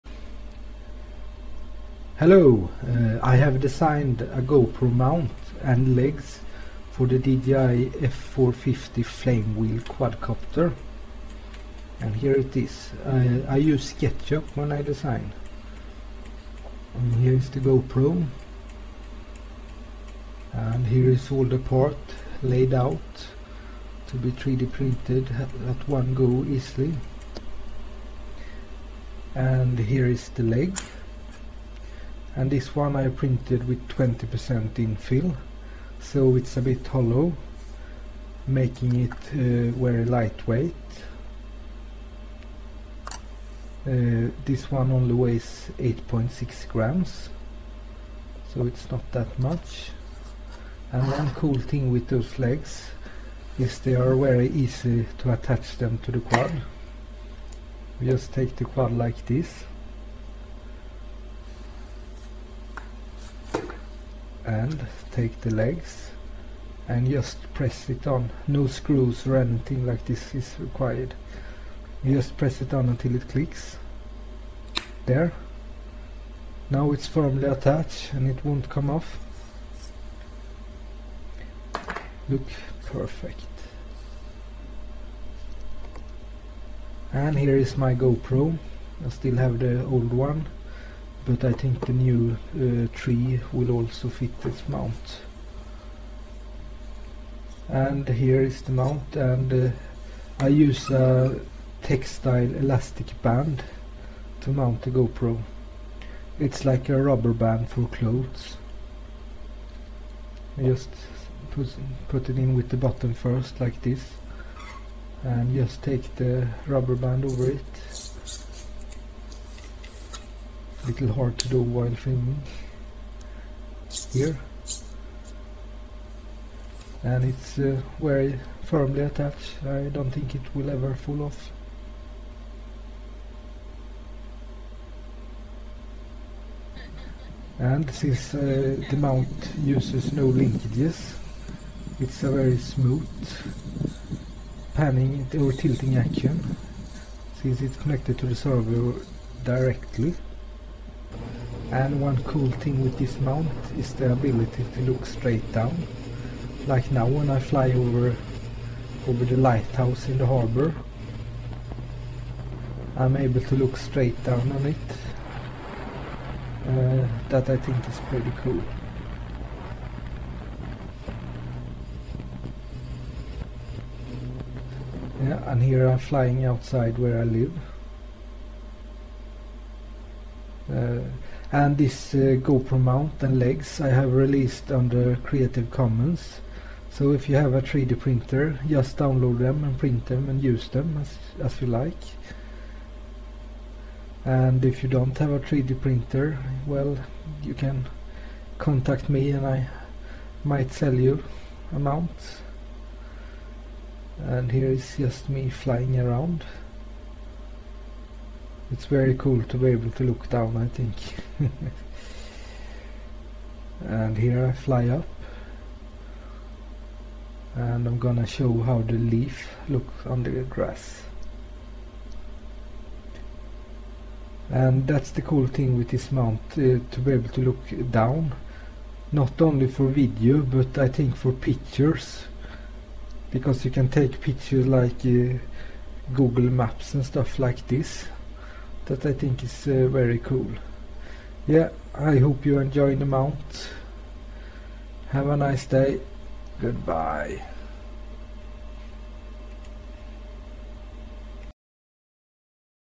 3D Printable GoPro mount & legs for the "Dji f450 flame wheel" quadcopter
I have designed a 3D printable GoPro mount and legs for the dji f450 quadcopter frame. With the ability to look straight down. Very minimalistic design, very lightweight. Uses one servo for tilt which is connected directly, no linkages, zero play, thats smooth tilt :-)